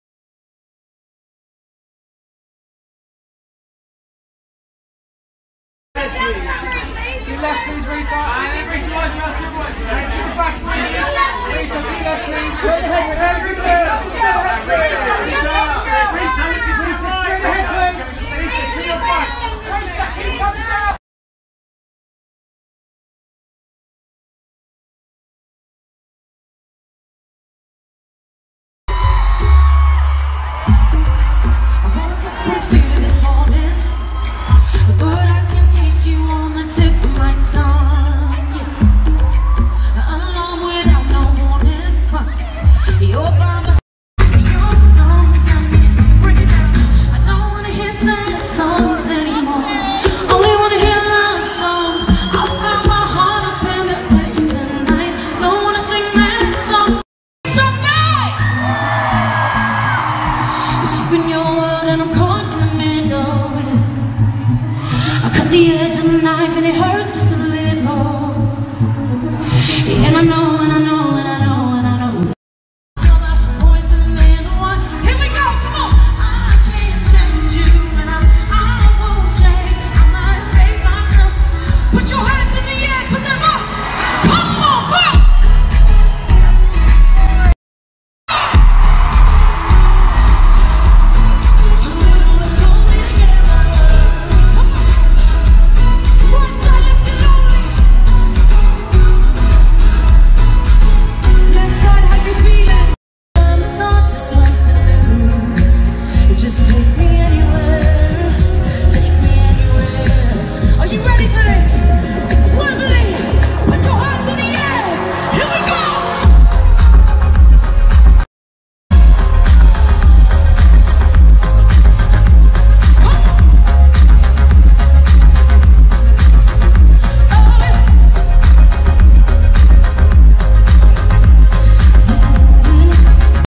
Rita Ora makes the perfect Poison Ivy at Kiss House Party
Rita Ora makes the perfect villain as Poison Ivy at Halloween Kiss House Party. Rita shined on the red carpet and also commanded the stage at the event.